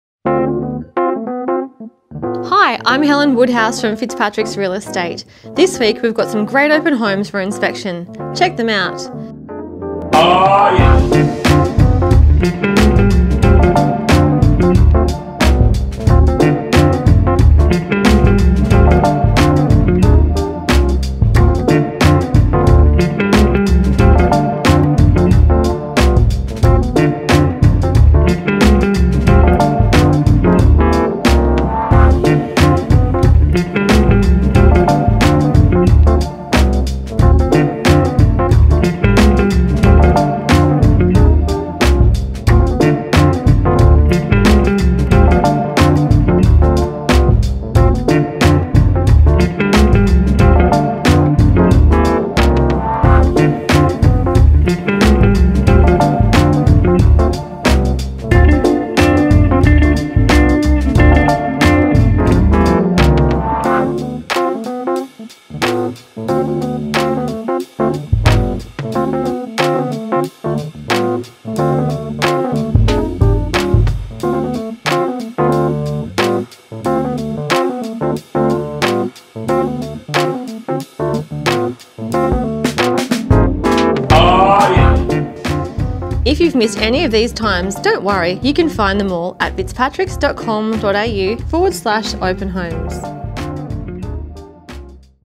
Open Homes - Saturday the 23rd February 2019
Hi it's Helen Woodhouse from Fitzpatricks Real Estate, while you're out and about this weekend be sure to check out some of these open homes.